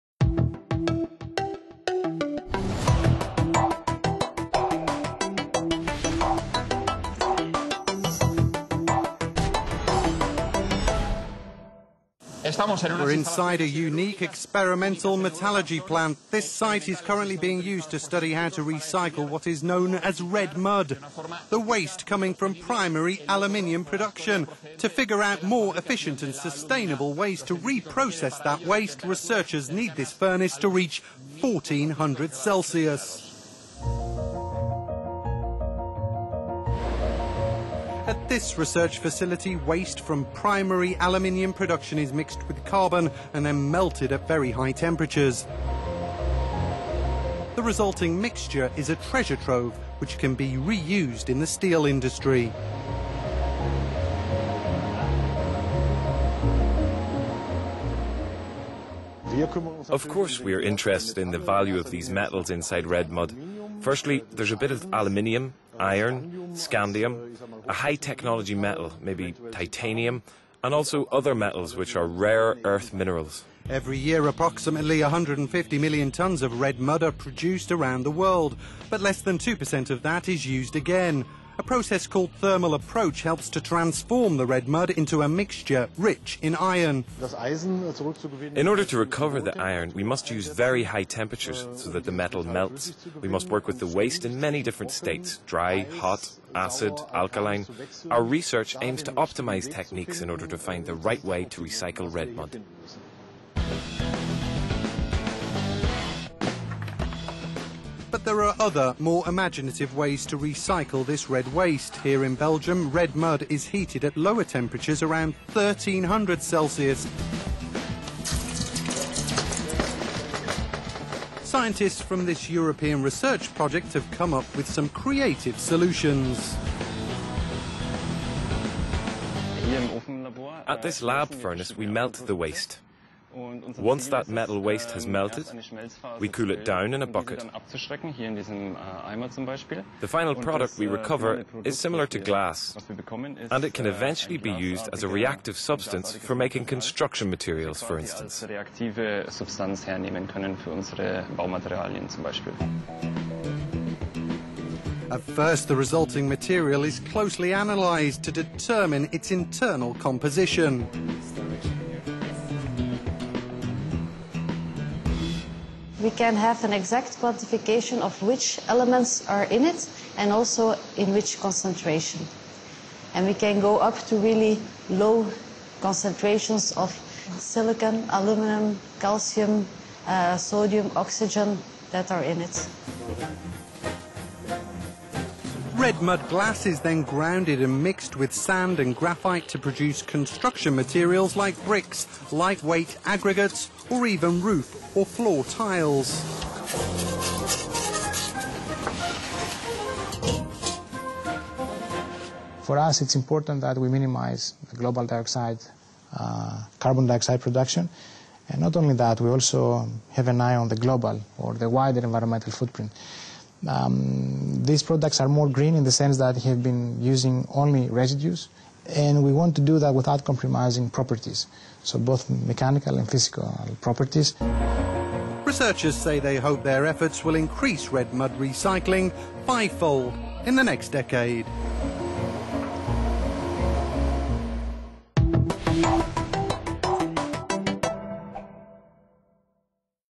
Aluminium production: what to do with 150 million tons of waste
Futuris meets scientists in Germany and Belgium hoping to dramatically improve ways to increase Red mud recycling.…